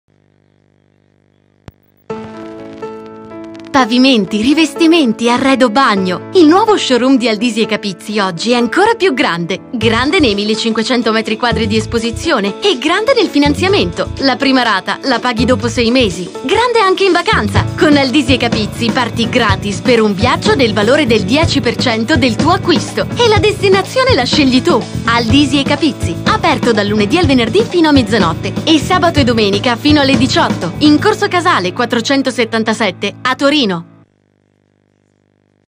SPOT Aldisi e Capizzi
Aldisi e Capizzi: pavimenti, rivestimenti, arredo bagno. il nuovo show room di Aldisi e Capizzi oggi è ancora più grande!
Spot realizzato da input/world